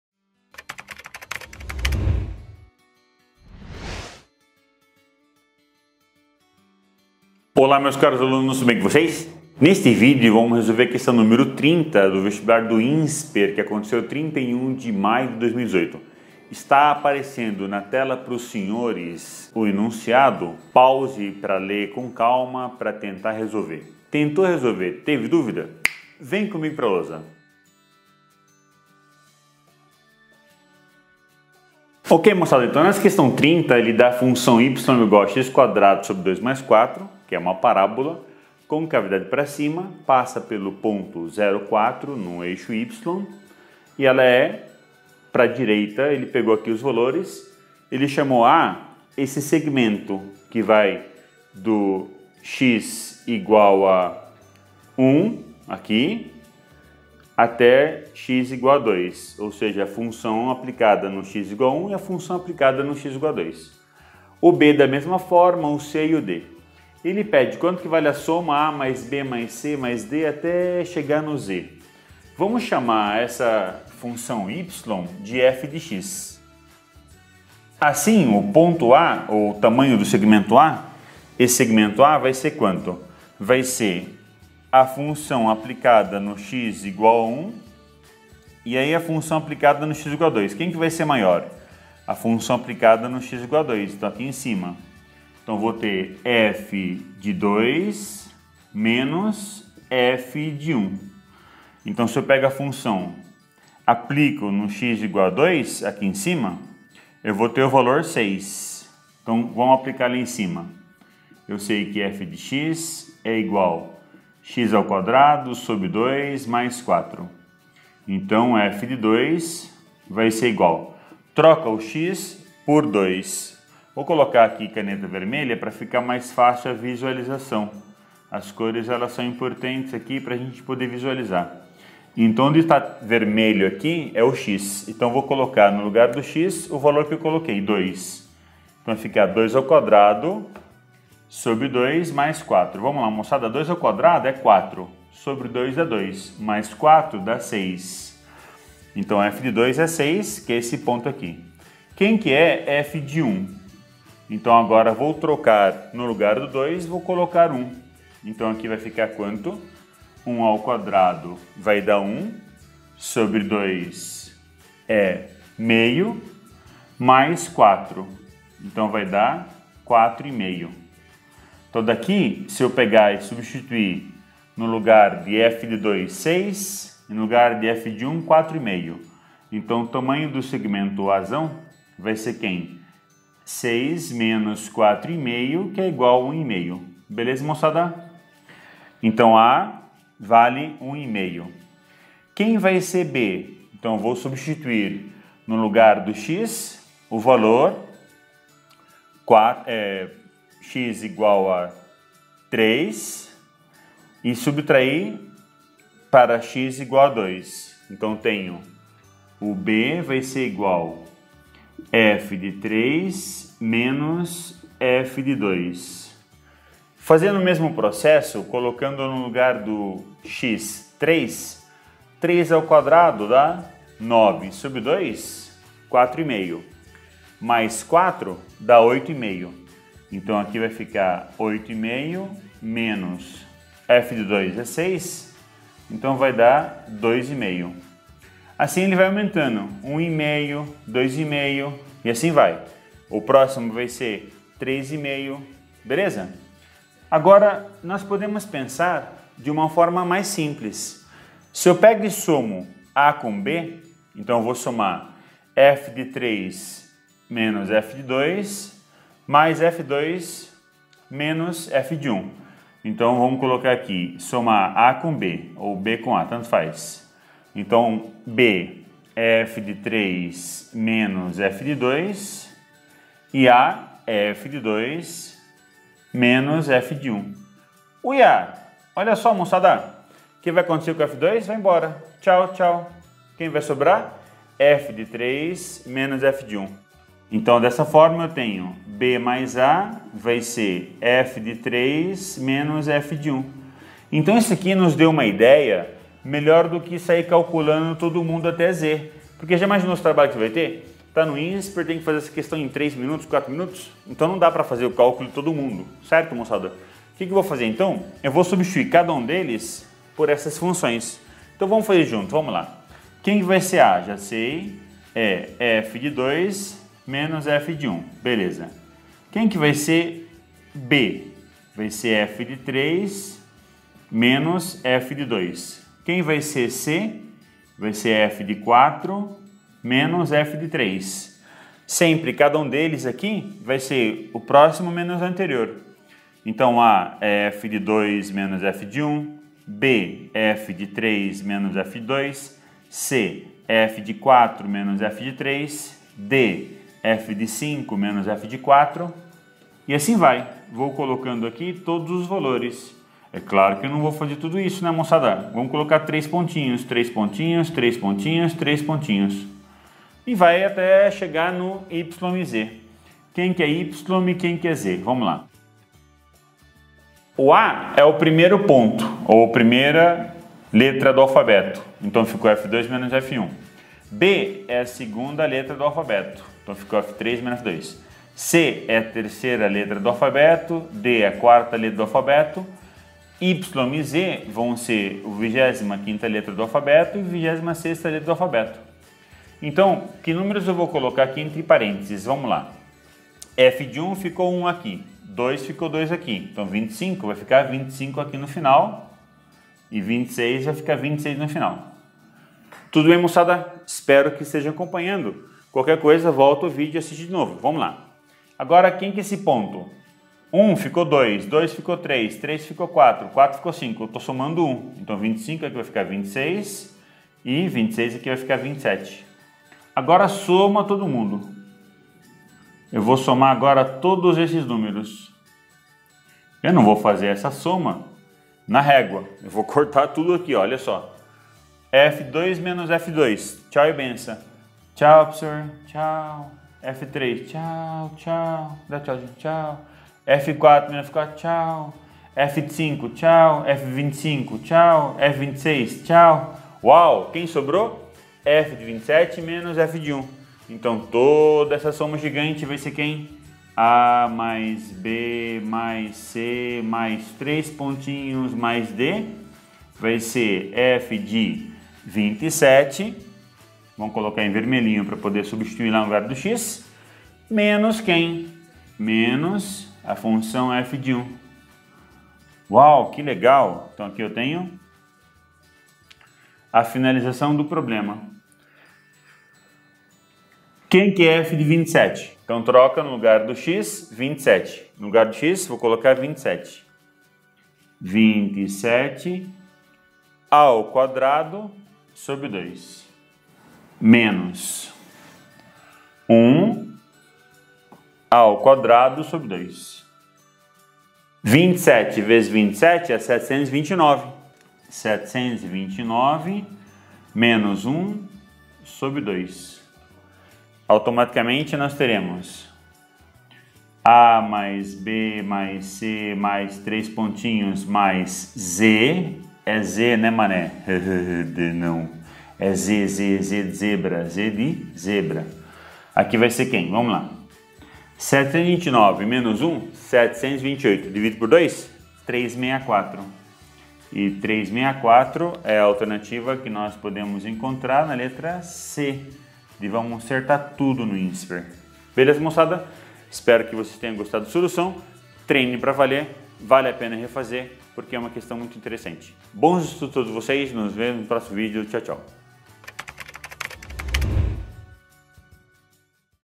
30 Matemática INSPER 2018 31/maio/2018
30 Matemática INSPER 2018 31/maio/2018

Questão 30
0 grafico, indica a fungao, quadratica, de IR em IR, dada, por,

y = x²/2 + 4. Nesse, grafico, os intervalos, horizontais indicados,

por 1, 2, 3 e 4 determinam, os intervalos verticais, indicados, por A, B, Ce D, respectivamente.
Mantendo-se o padrao descrito, e considerando, o alfabeto, de 26 letras, a soma, A+ B + C + D + E + ... + Z equivale, a um segmento, de medida, igual a
(A) 398.
(B) 456.
(D) 484.
(E) 414.